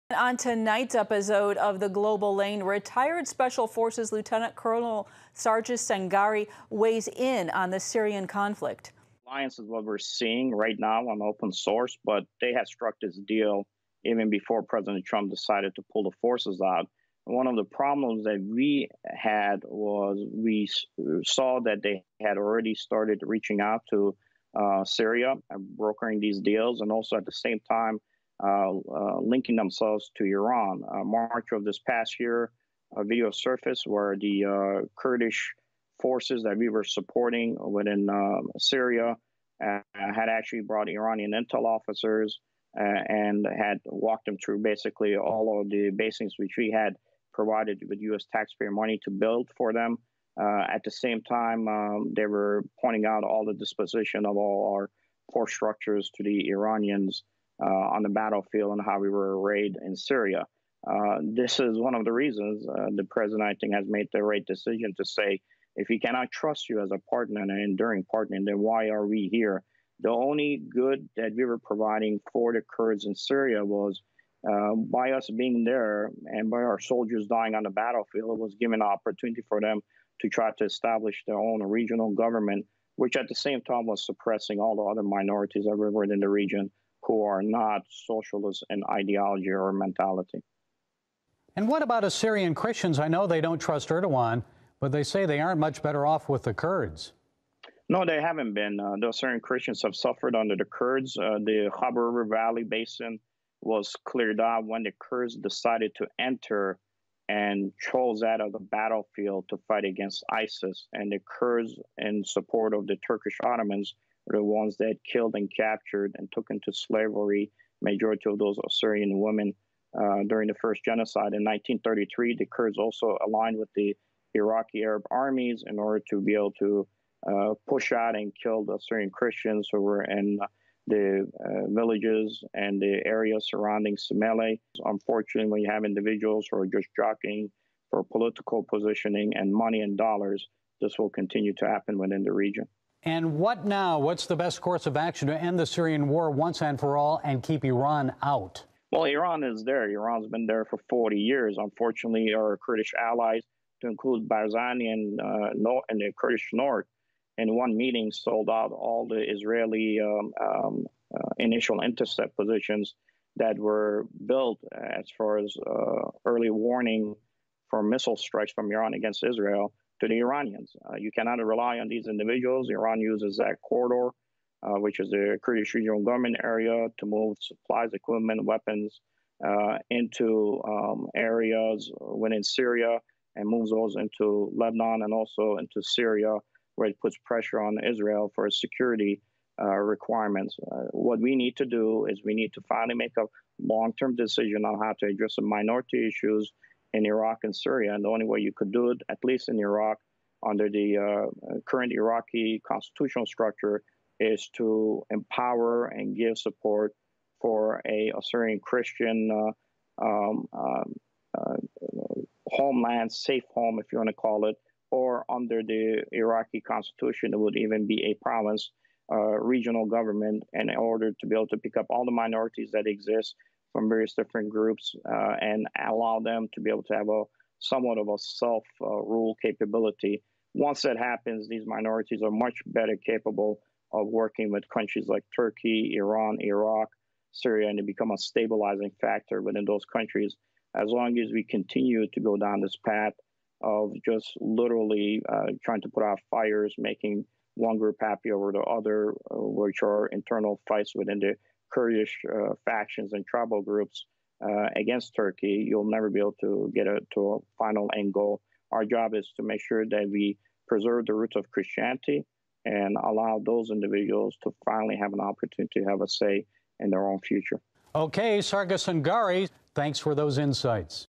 17 OCT 19, Interview of LTC Sargis Sangari, CEO NEC-SE, by Gary Lane on CBN
On 17 OCT 19, LTC Sargis Sangari, CEO NEC-SE, was interviewed by Gary Lane on Christian Broadcasting Network (CBN). The subject of the discussion focused on POTUS decision to withdraw from Syria, the Kurdish actions leading to the decision, their linkages to Iran and the Syrian government, and the Kurdish historical animosities with the minorities.